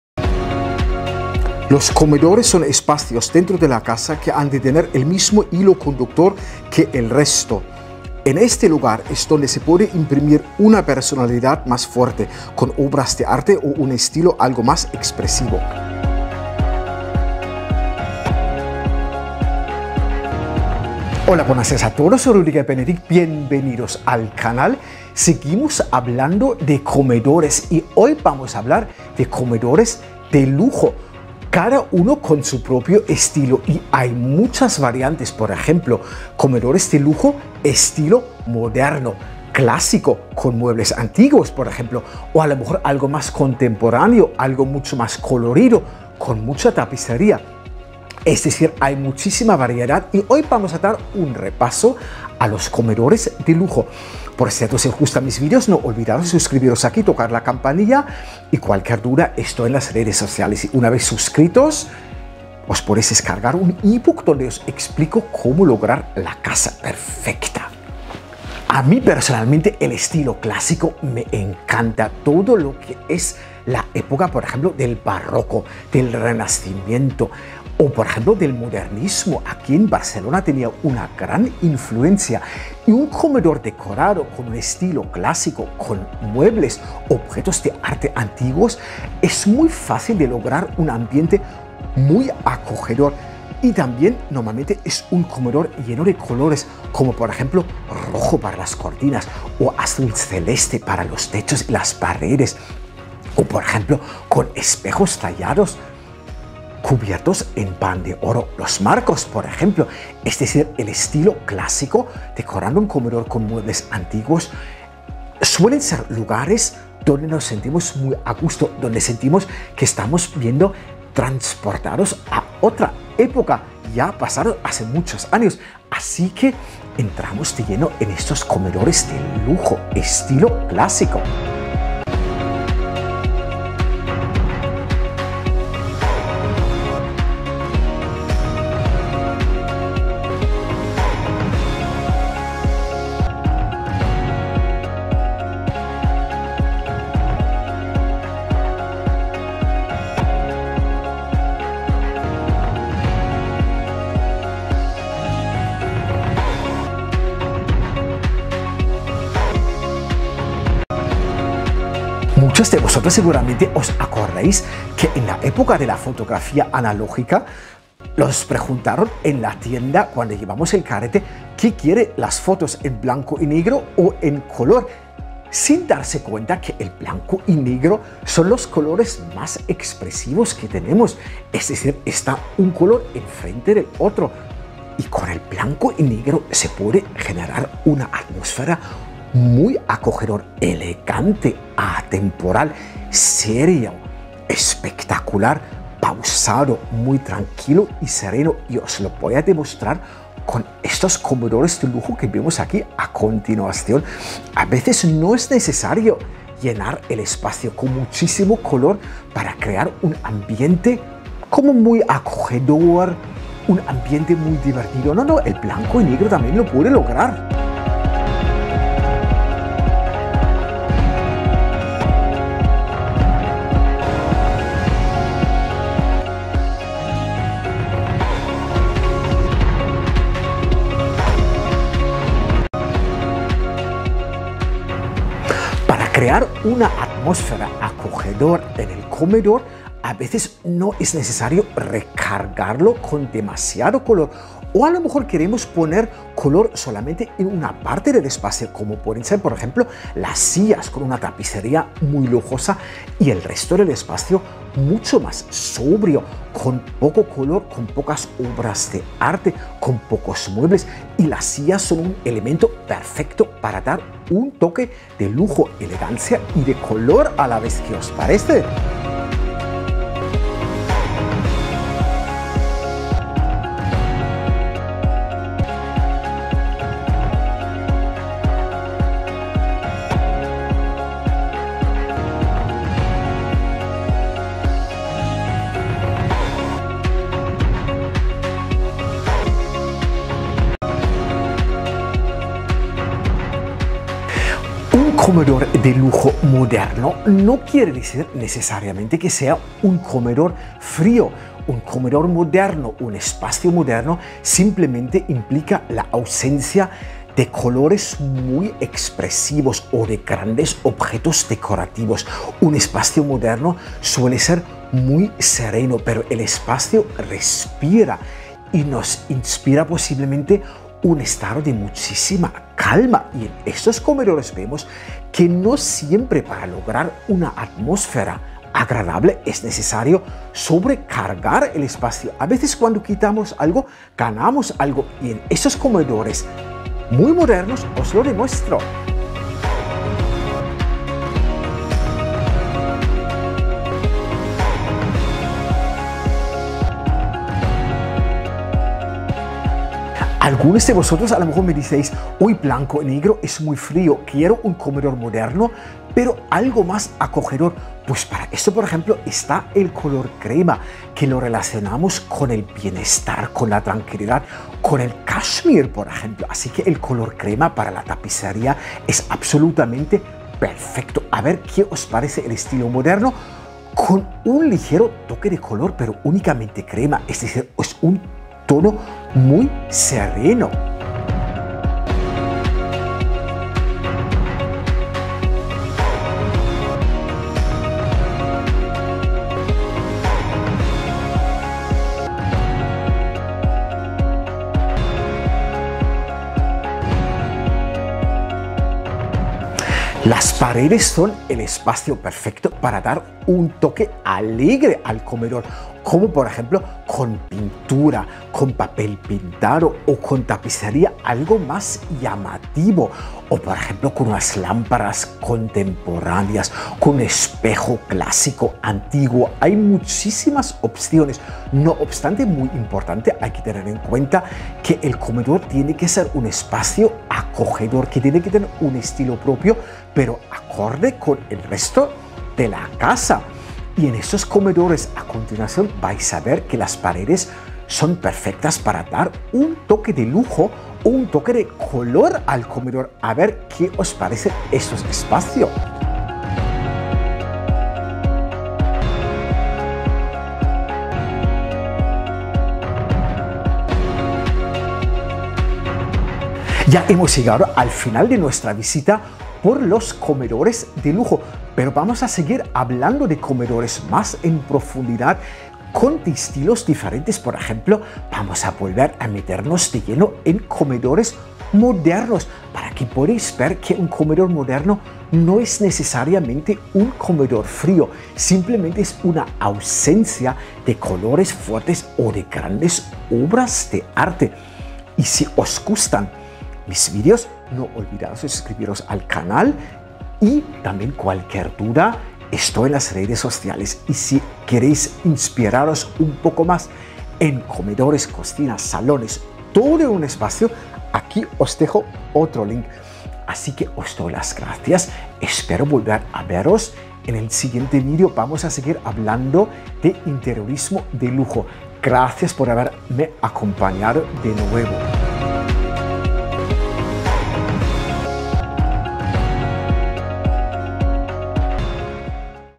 ¡LAS TENDENCIAS MÁS IMPORTANTES PARA COMEDORES DE LUJO!
👈 Entra ahora ✅ ¿Te interesa la decoración de los comdores de lujo? ¿Quieres saber más sobre las últimas tendencias de los comedores lujosos para este año 2024?

Pues quédate a ver este vídeo, porque no hay mejor forma de informarte sobre las novedades en los salones de casas de diseño actual que disfrutar de este video.

Partes del vídeo:
01:50 Tendencia 1:Tendencias para comedores
03:28 Tendencia 2: Mesas para el comedor
05:16 Tendencia 3: Mobiliario para comedores de lujo0
6:40 Tendencia 4: Colores para comedores de diseño

Normalmente cuando hablamos de nuevas tendencias en el mundo del interiorismo, observamos novedades en las casas en general.Pero cada estancia dentro de nuestra casa es un mundo, las tendencias de las cocinas actuales no tienen nada que ver con la actualidad de los salones lujosos modernos.

Estamos viendo esta temporada que los comedores de lujo se visten de las mejores galas veraniegas, con tejidos naturales y frescos, con tonos muy claros. Y también los elementos funcionales como las mesas de comedor han ido evolucionando, ahora las vemos en tamaño extra-grande y tapiado en tejidos.

Los muebles vienen con mucha discreción, con tonos suaves, sin destacar.Y a la hora de elegir un tono general para un salón con diseño de lujo, el color crema es la mejor opción. Pero mejor que te quedes a disfrutar del video entero, así estarás al día de las últimas tendencias en el mundo de la decoración.
Pero no obstante, para lograr un comedor de diseño de lujo hay que tener en cuenta el concepto del diseño de la casa en general. Cuando diseño una casa con un estilo muy actual, me suelo enfocar en una decoración de comedor con elementos más bien actuales. La madera natural nunca falla, pero a la vez una alfombra con un diseño algo más llamativa siempre es un punto muy a favor.

El comedor de lujo es el espacio perfecto dentro de nuestra casa para agregar algo de color, por lo tanto una obra de arte de diseño abstracto es el punto focal perfecto.